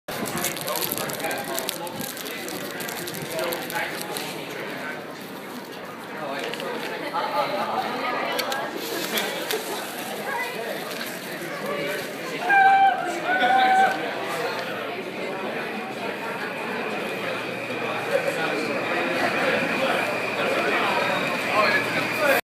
Stuck on a train day
Train broke down for half an hour then they piled us on a ramp at the next station to switch us all onto a new train.